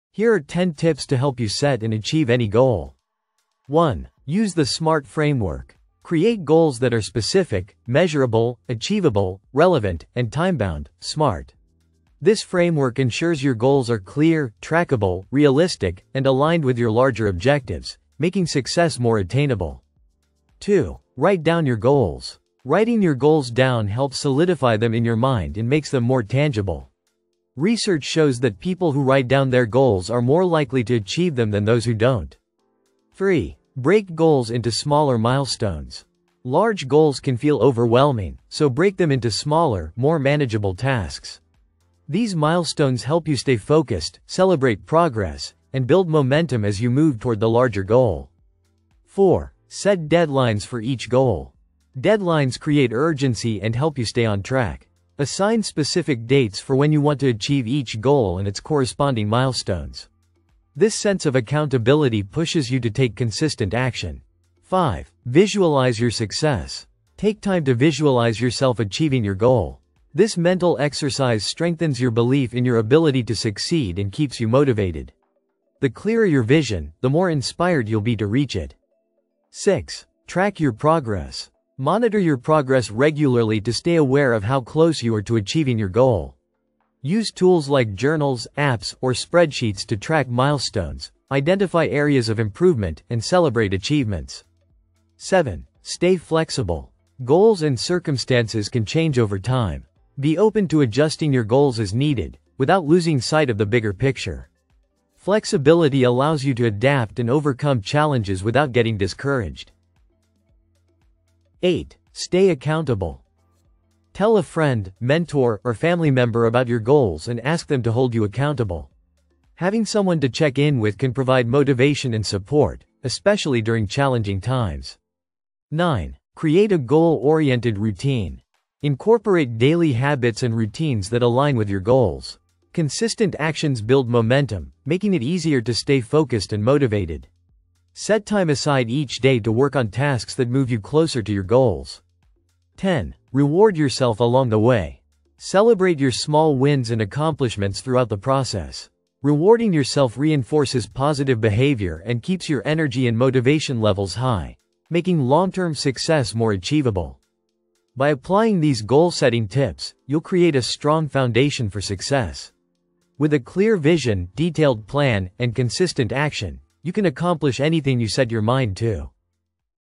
Top 10 Goal Setting Tips to Help You Achieve Anything
Master the art of goal-setting with these essential tips. Learn how to set and achieve any goal by applying practical strategies and maintaining focus.